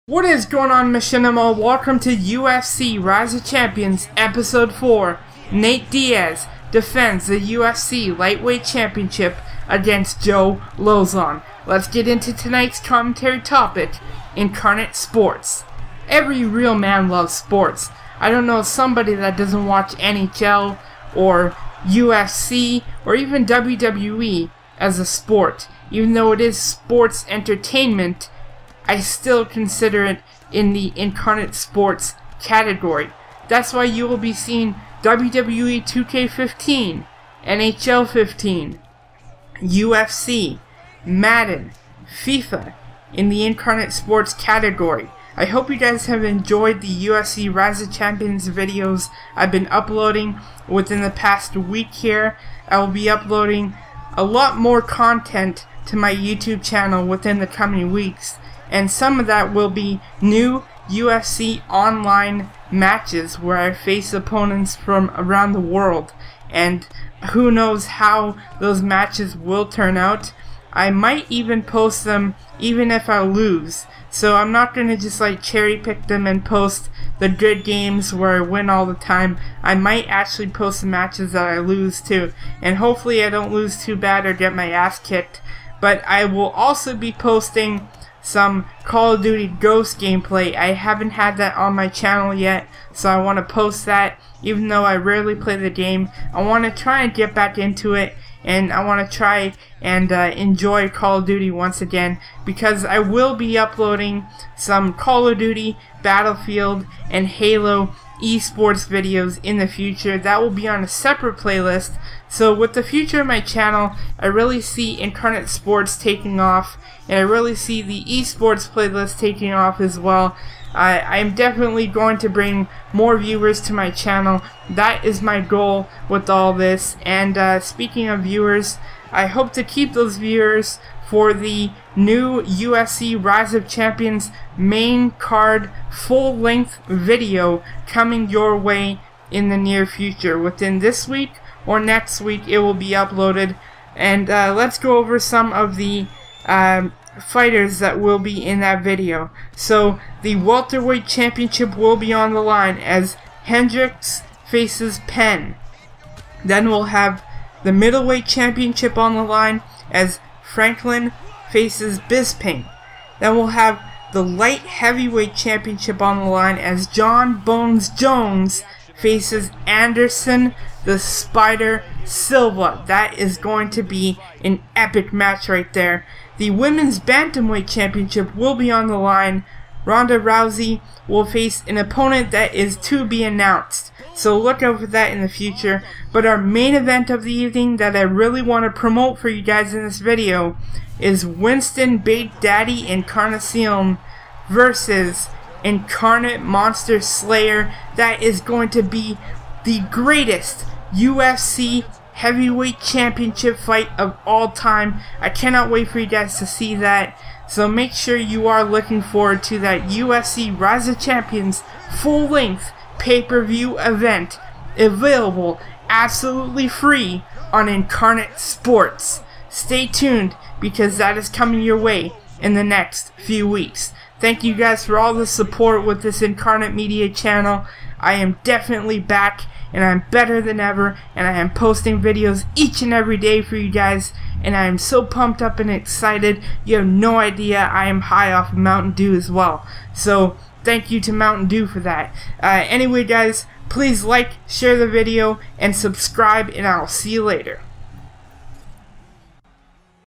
Incarnate Sports - UFC Rise of Champions Episode 4
Nate Diaz defends the UFC lightweight championship against Joe Lauzon!

UFC Rise of Champions is a new video series kicking off Incarnate Sports. In this video series every UFC Championship will be defended in a five round title match. Use the UFCROC on twitter to stay updated with new videos.